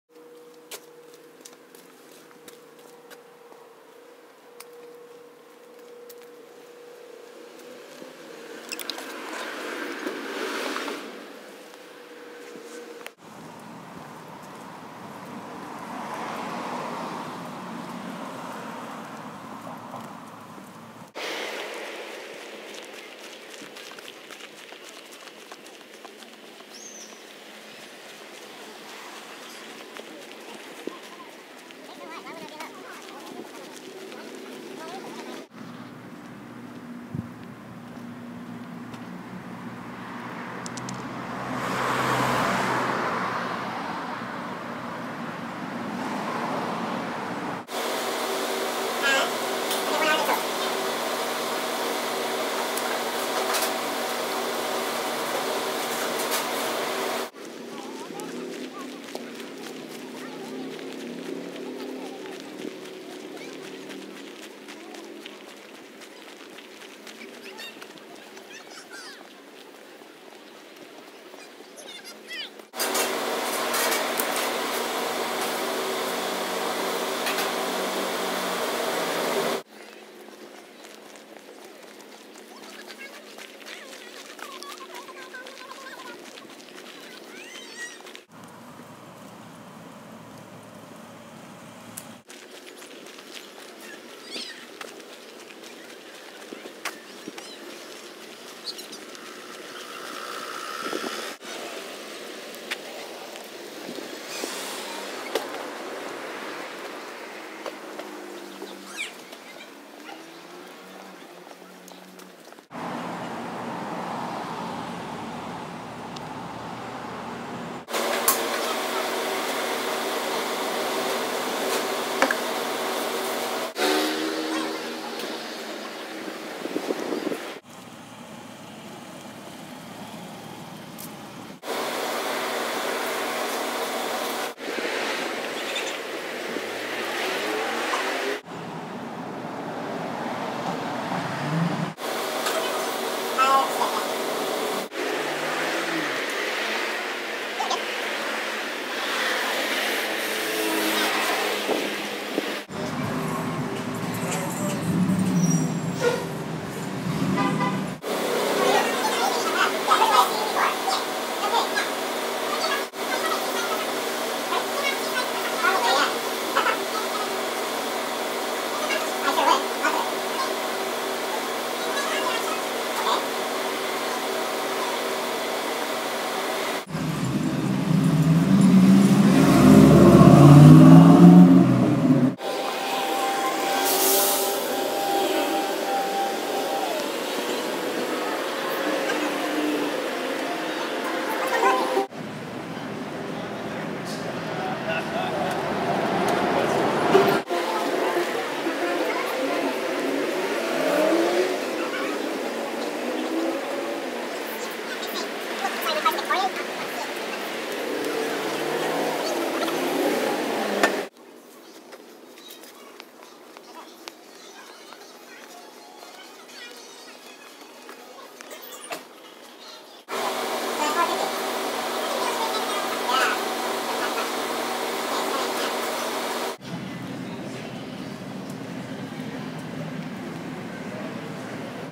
Sunday On The Drive Redux Mobile
A compilation of bits of the footage I took walking around my neighbourhood today. Some of the footage is sped up, all of it, shot with the ipad app Cartoonatic